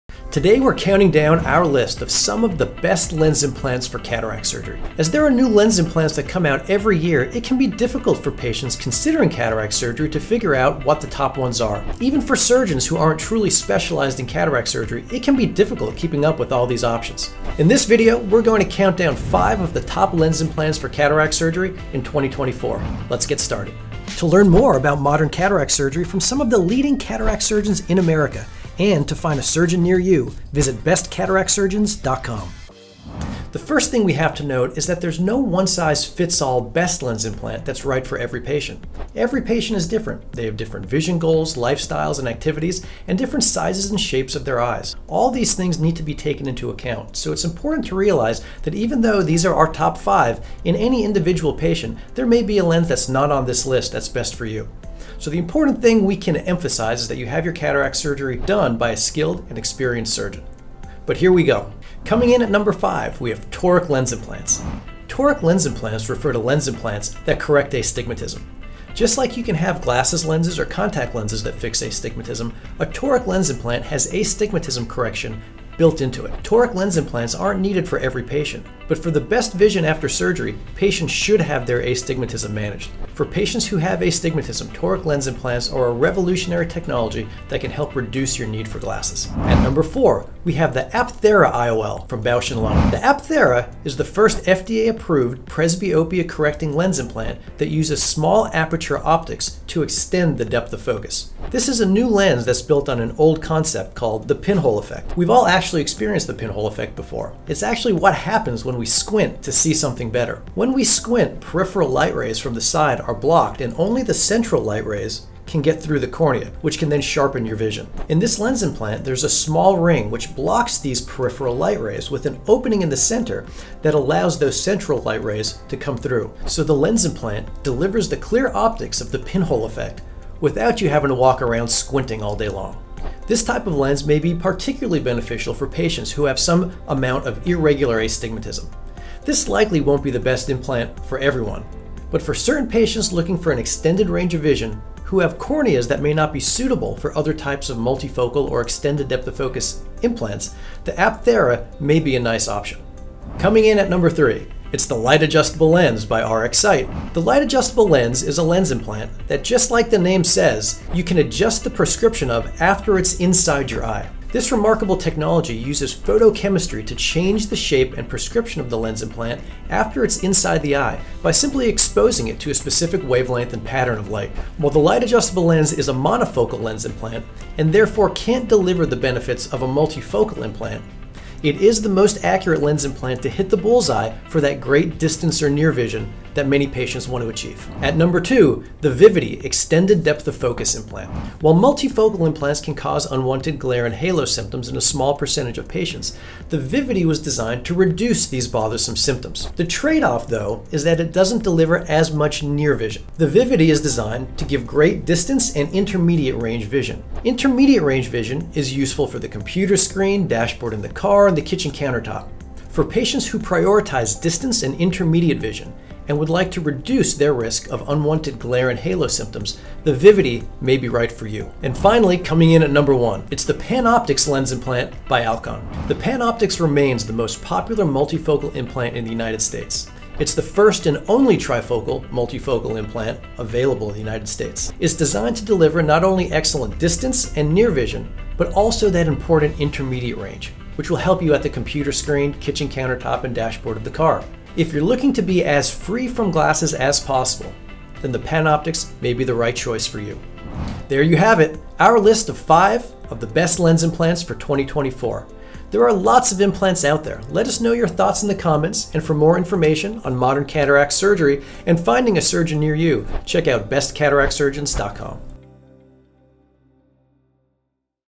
Best Lens Implants for Cataract Surgery in 2024
The best lens implant for cataract surgery is a personal decision based on the health of your eyes, your vision goals, and your lifestyle. Every patient is different, and a personalized, in-depth discussion with an experienced surgeon is important to choose the best lens implant for your eyes.

In this video, we'll countdown 5 of the most popular lens implants for cataract surgery in 2024.

0:00 - Introduction
0:33 - Background / Every Patient is Different
1:03 - Number 5
1:29 - Number 4
2:34 - Number 3
3:08 - Number 2
3:42 - Number 1
4:11 - Summary

Featured Surgeon:
Dr. Michael Shumski, MD - Orlando, FL